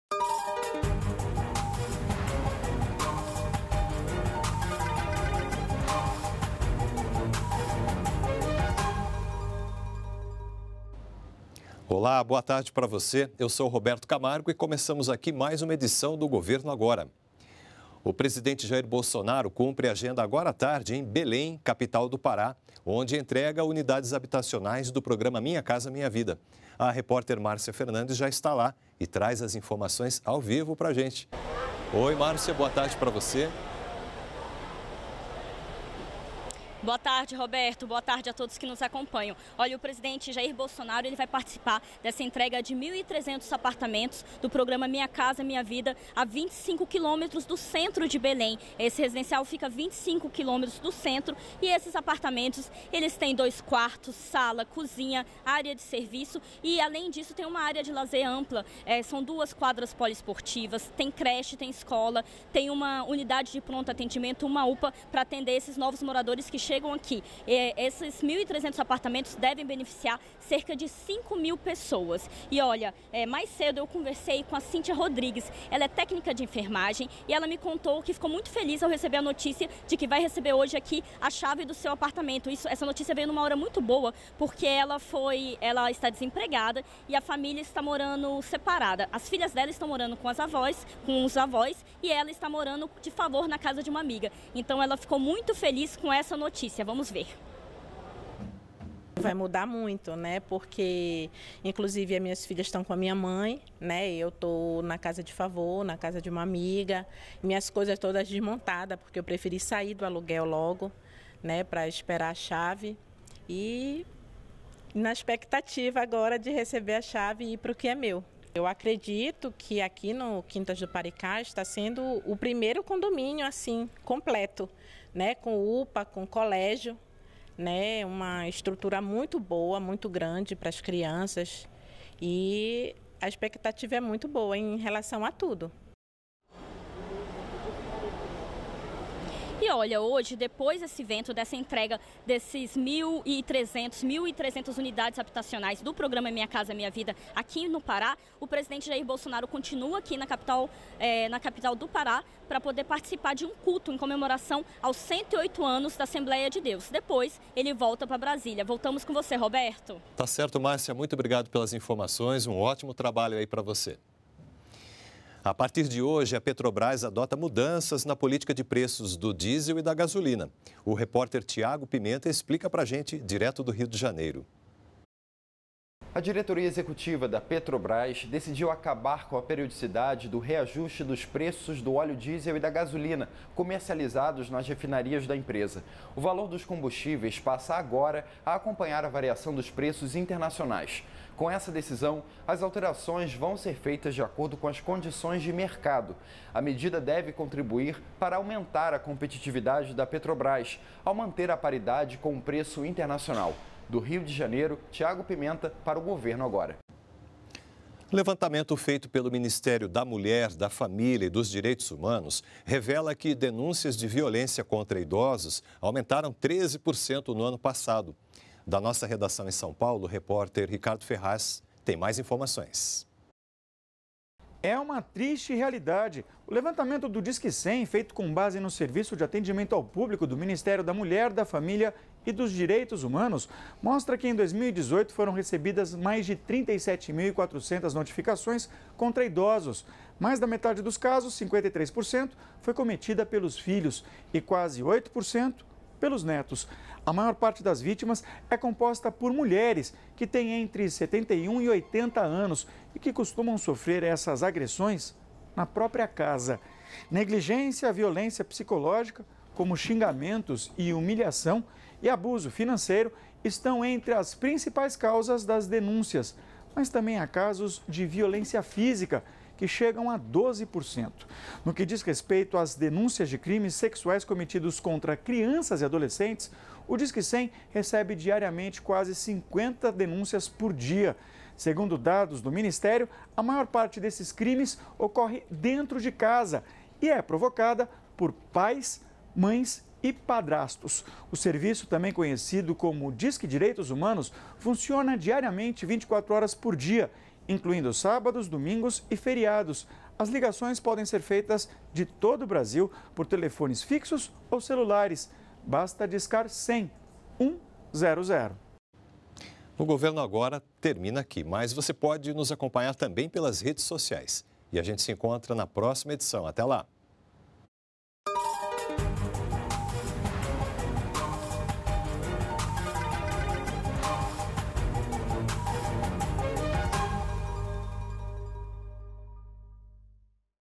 Minha Casa, Minha Vida irá entregar 1300 apartamentos em Belém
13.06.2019 - O presidente Jair Bolsonaro cumpre agenda em Belém, capital do Pará, onde entrega unidades habitacionais do Programa Minha Casa, Minha Vida. Veja também: Levantamento do Ministério da Mulher, da Família e dos Direitos Humanos revela que denúncias de violência contra a idosos aumentaram 13% em 2018.